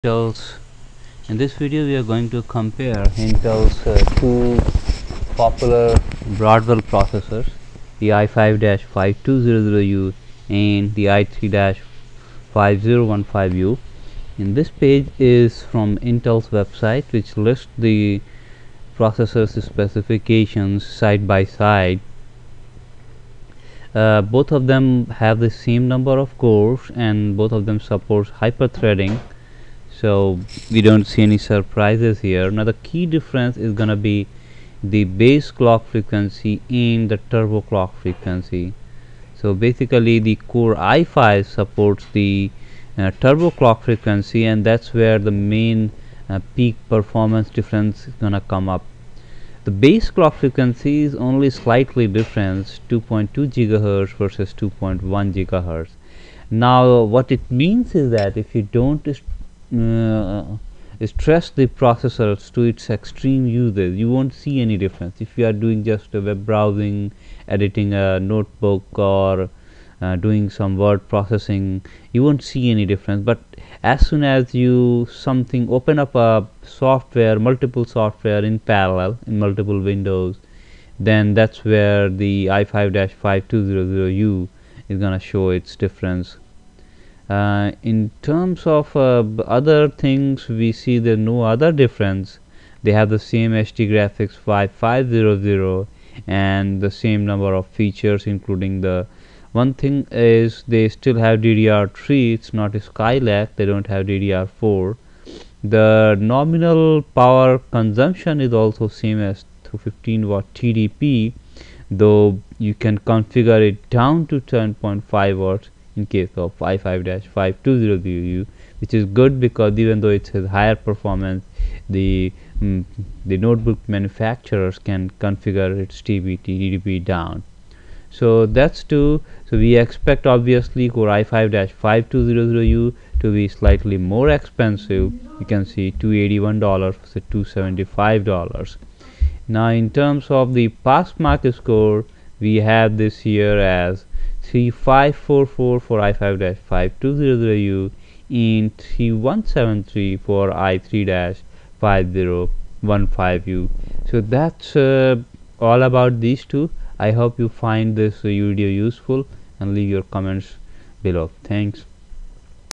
i3 5015u vs i5-5200u
i5-5200u has 100 MHz higher clock frequency and supports turbo clock making it about 10% faster in peak performance.